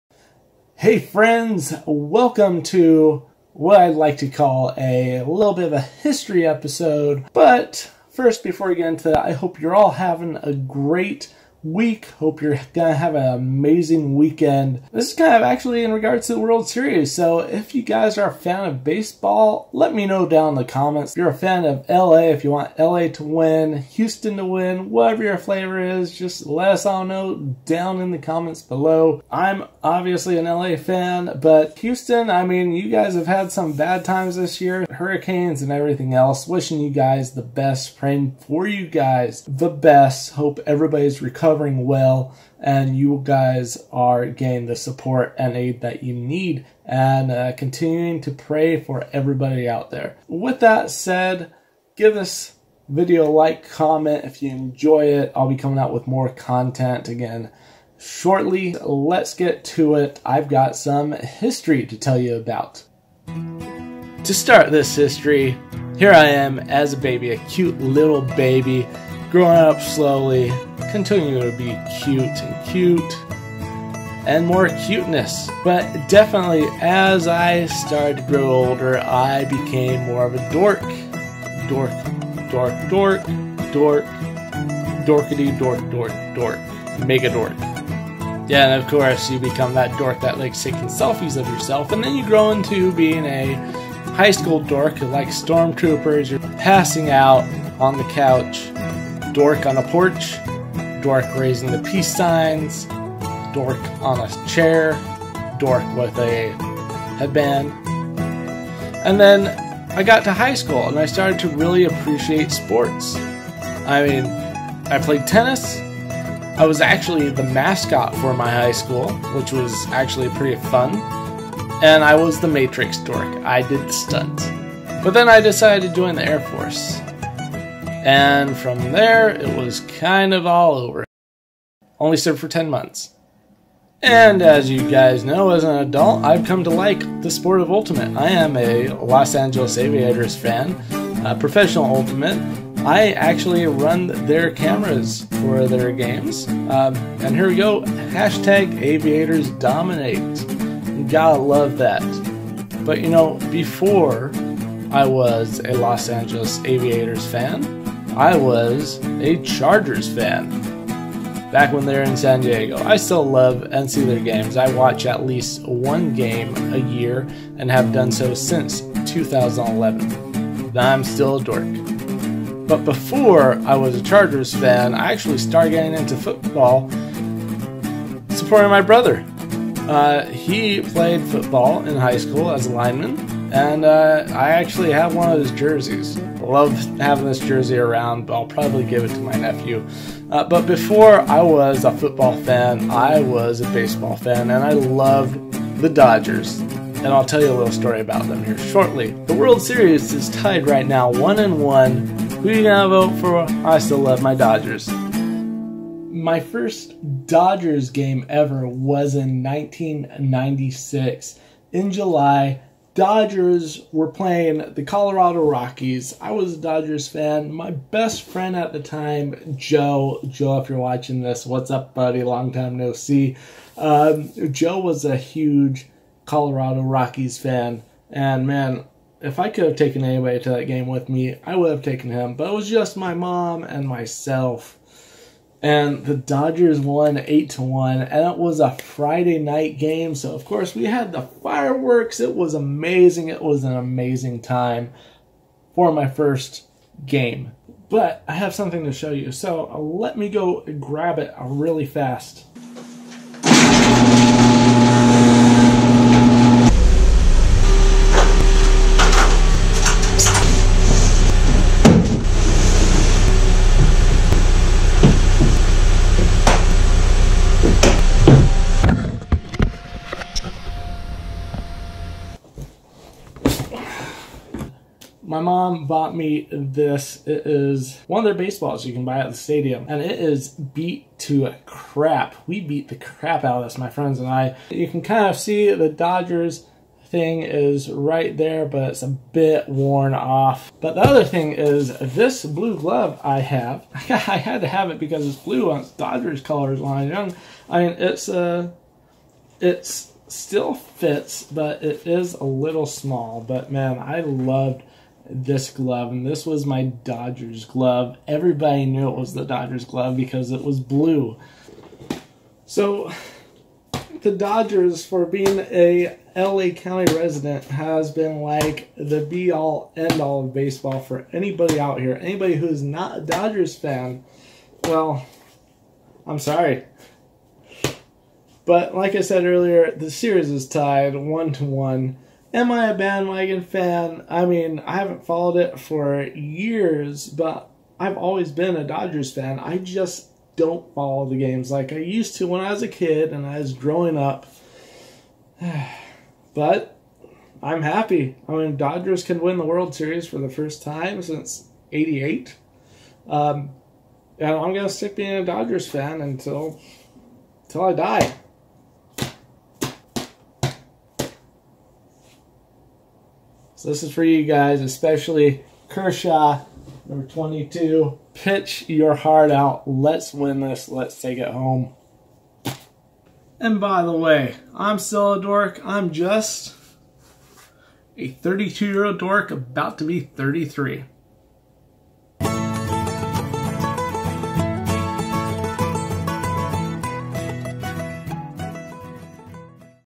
Dodgers Bandwagon Fan
Who are you a fan of? Am I a Dodgers bandwagon fan? So let me tell you about my past love of sports, and you can decide.

Instagram and Snapchat @lanceilievvideo
Twitter (whenever I do) @lanceiliev